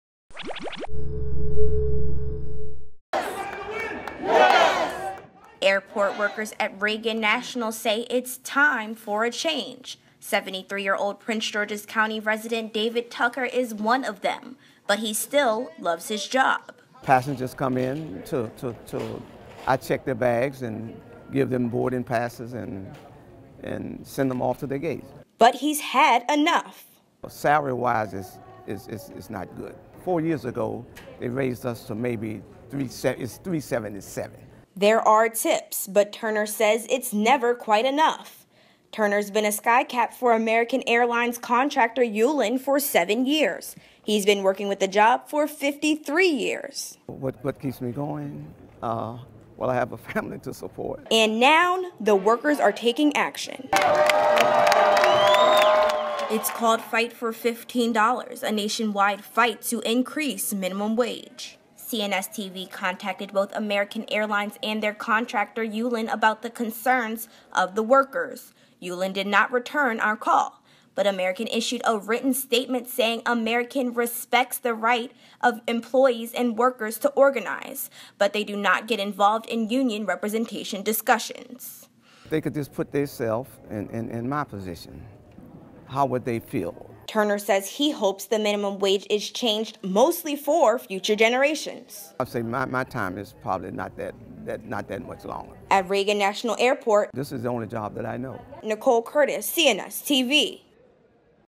AIRPORT LABOR: Workers fight for more pay
ARLINGTON -- Imagine working every day for under four dollars an hour. That's the situation for many airport workers in the metro area and around the rest of the country.